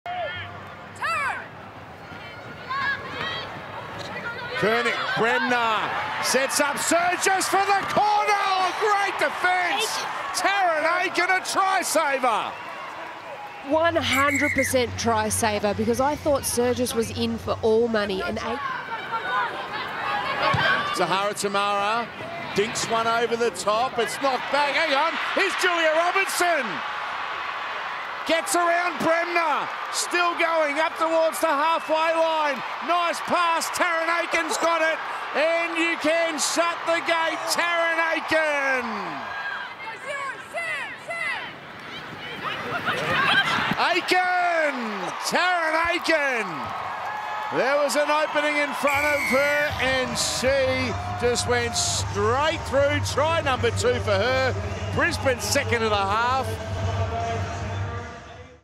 Tarryn Aiken had a day out against the Roosters. 👏
Tarryn Aiken was in everything good for the Brisbane Broncos against the Sydney Roosters in Round 2 of the 2022 NRL Telstra Premiership.